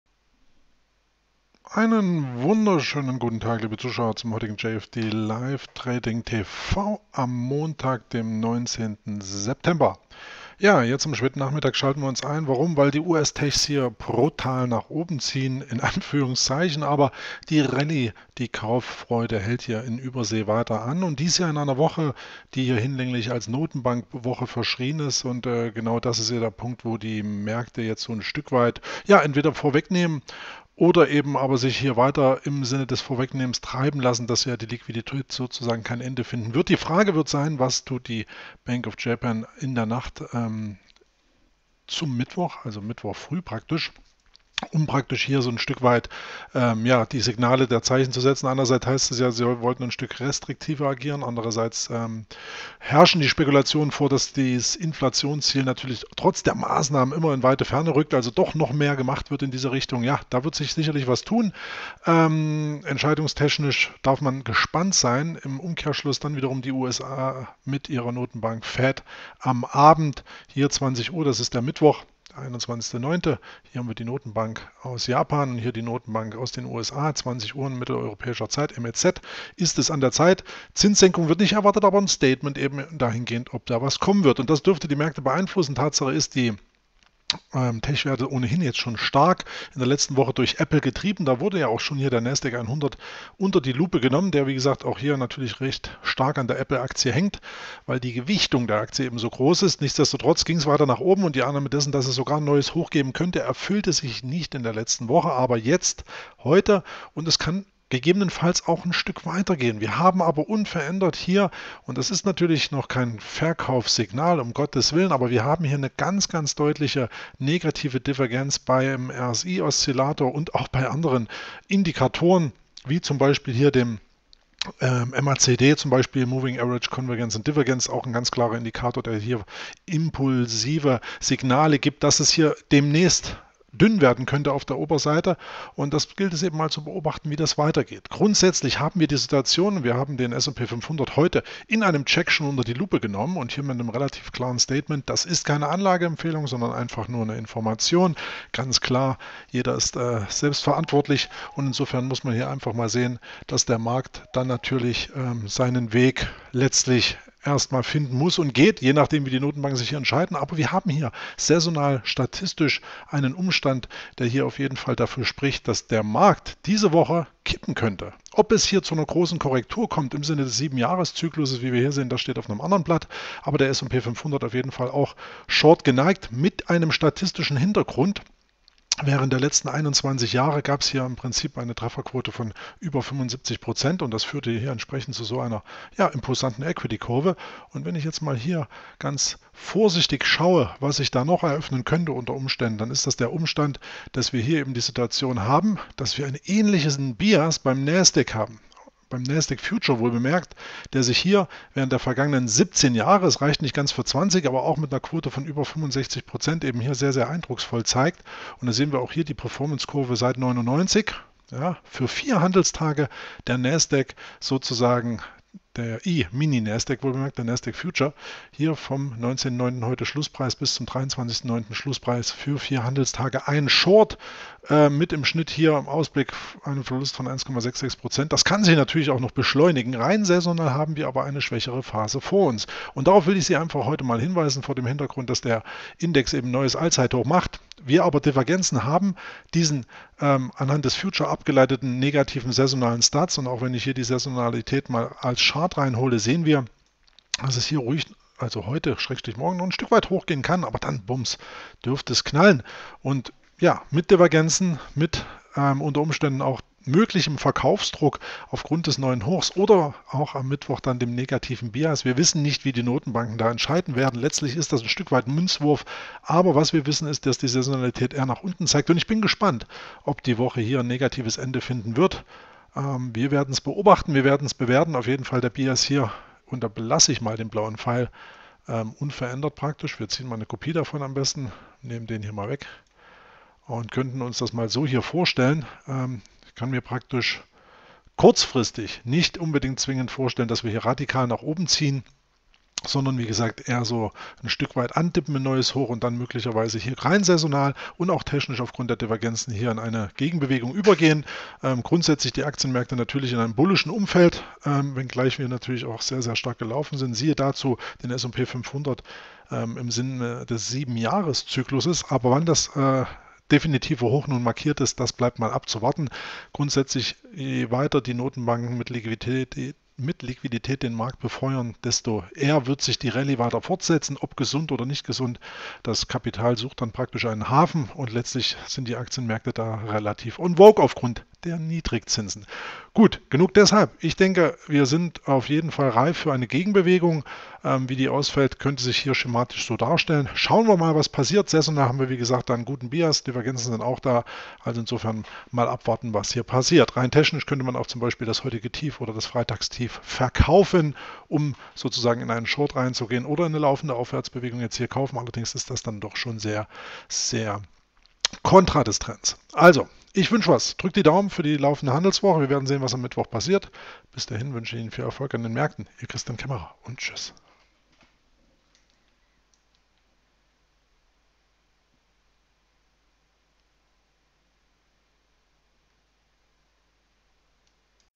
Nasdaq100 mit neuem Rekordhoch – Gefahr?!
Der Nasdaq100 bleibt auf Kurs und markiert just heute zum Wochenauftakt ein neues Allzeithoch. Technisch und saisonal gibt es jedoch unveränderte Warnsignale, sodass man auf der Hut.